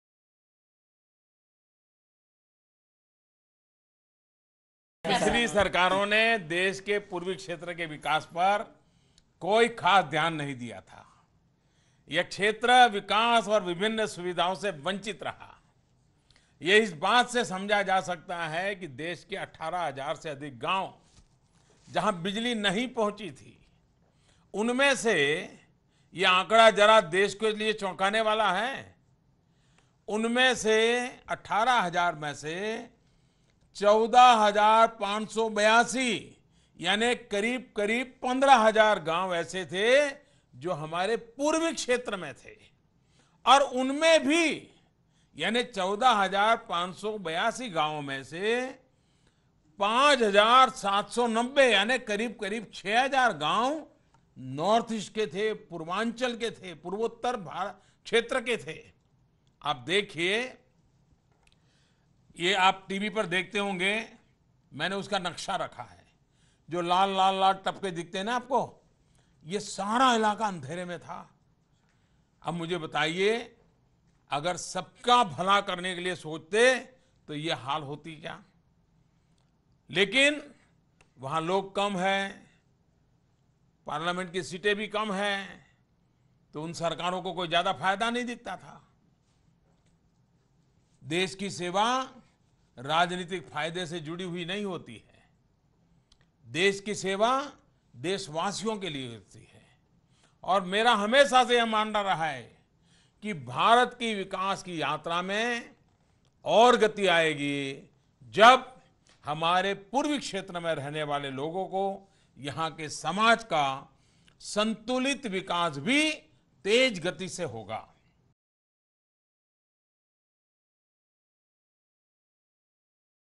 About 14,500 villages out of 18,000 villages not electrified were in Eastern India: PM Modi
New Delhi, Jul 19 (ANI): While interacting with the villagers across the country through video conferencing, Prime Minister Narendra Modi said, “One of the priorities of our government is the development of Eastern India. About 14,500 villages out of 18,000 villages were not electrified in Eastern India. We have changed that. Eastern India can play an even bigger role in India’s development journey”.